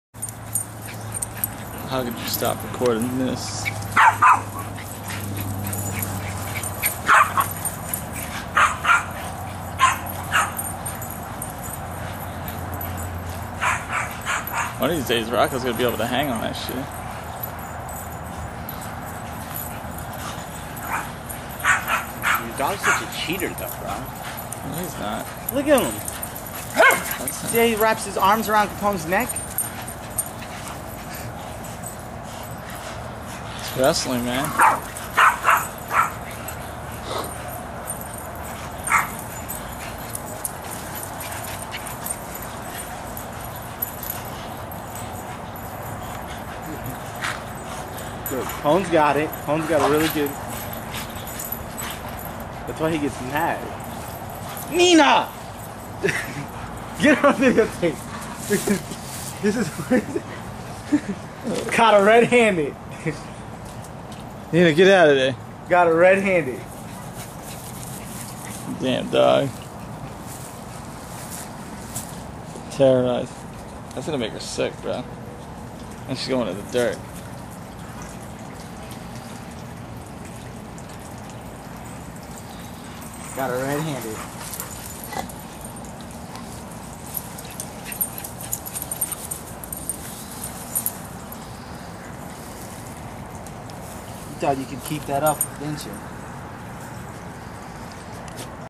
Pitbulls Playing with the rope.
Our dogs having some good ole backyard fun.